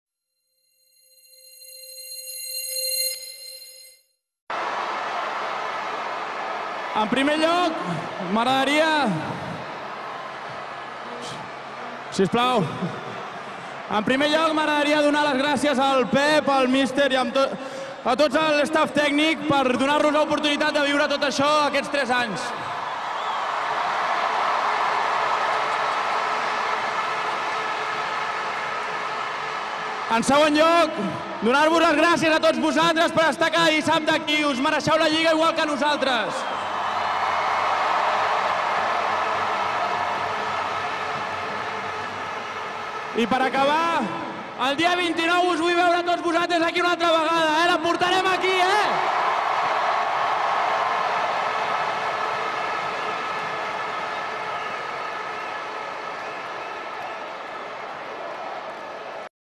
Gerard Pique - FC Barcelona Celebration of La Liga 2010 - 2011 in the Camp Nou
Gerard Pique in the Camp Nou.

"First I'd like to thank the staff for all their work over the three years. Secondly, to thank all of you for turning up here every Saturday -you deserve the title as much as we do. Finally, I want to be back here to see you all on 29th! We'll bring it back".